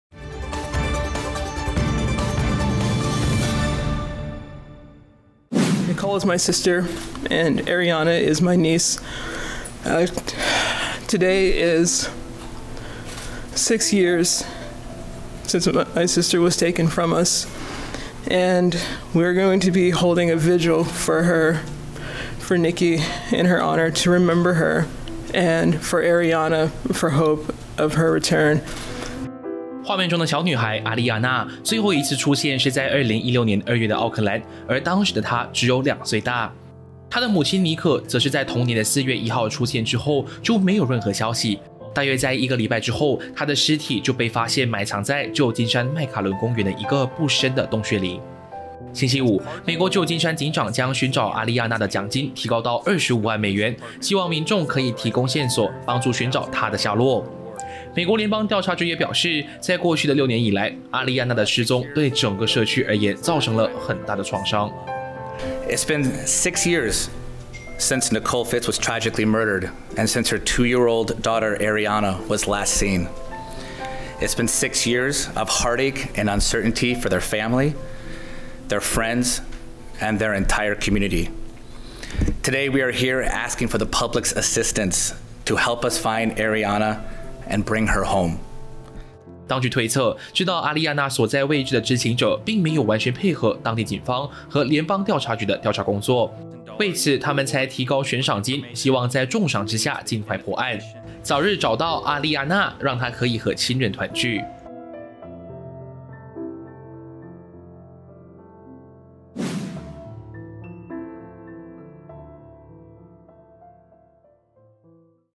母被杀2岁女失踪6年 美警悬赏25万美元寻人
美国一小女孩在2016年失踪至今仍无下落，警方提高悬赏金至25万美元，希望民众能帮忙寻找她。